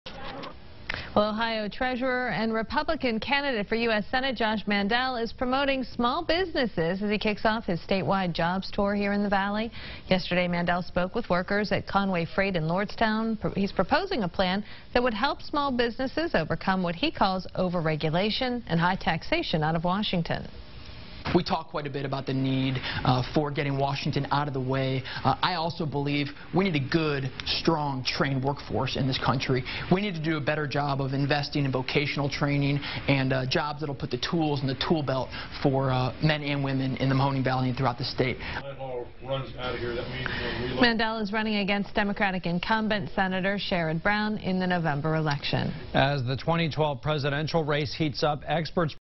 Josh Mandel Tours Con-Way Freight In Lordstown And Discusses His Jobs Plan -- FOX Youngstown
Ohio Treasurer Josh Mandel toured Con-way Freight in Lordstown. The visit from the Senate candidate was part of his 22 county Jobs Tour across the Buckeye state. Be sure to become a Fan of Josh on Facebook and follow him on Twitter @JoshMandelOhio or the campaign @Josh4Senate